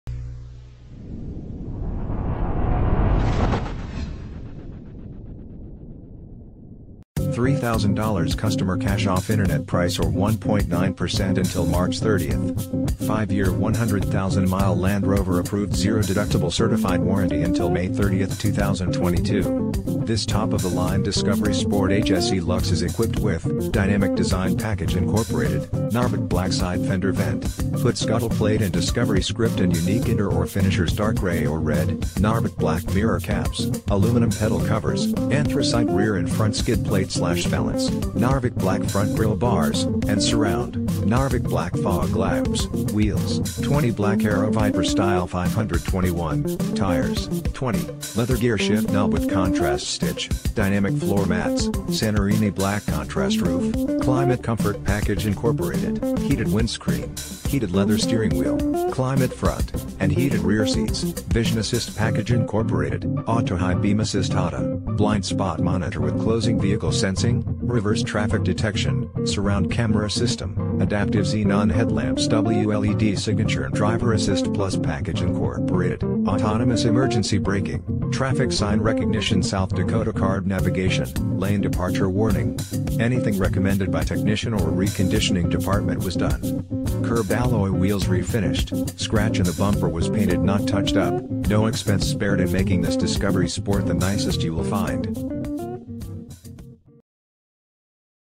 2017 Land Rover Discovery Sport Minneapolis, Golden Valley, St Paul, MN R820L
2017 Land Rover Discovery Sport HSE Luxury

$3000 CUSTOMER CASH OFF INTERNET PRICE OR 1.9% UNTIL MARCH 30TH!! 5 year 100,000 mile Land Rover Approved zero deductible certified warranty until May 30th 2022! This top of the line Discovery Sport HSE LUX is Equipped with; DYNAMIC DESIGN PACKAGE-inc: Narvik black side fender vent, hood scuttle plate and Discovery script and unique interor finishers (dark gray or red), Narvik Black Mirror Caps, Aluminum Pedal Covers, Anthracite Rear & Front Skid Plate/Valance, Narvik Black Front Grille Bars & Surround, Narvik Black Fog Lamps, Wheels: 20 Black Aeroviper Style 521, Tires: 20, Leather Gear Shift Knob w/Contrast Stitch, Dynamic Floor Mats, SANTORINI BLACK CONTRAST ROOF, CLIMATE COMFORT PACKAGE-inc: heated windscreen, Heated Leather Steering Wheel, Climate Front & Heated Rear Seats, VISION ASSIST PACKAGE-inc: auto high beam assist (AHDA), Blind Spot Monitor w/Closing Vehicle Sensing, reverse traffic detection, Surround Camera System, Adaptive Xenon Headlamps w/LED Signature and DRIVER ASSIST PLUS PACKAGE-inc: autonomous emergency braking, Traffic Sign Recognition, SD Card Navigation, Lane Departure Warning. Anything recommended by technician or reconditioning department was done. Curbed alloy wheels refinished, scratch in the bumper was painted not touched up, no expense spared in making this Discovery Sport the nicest you will find!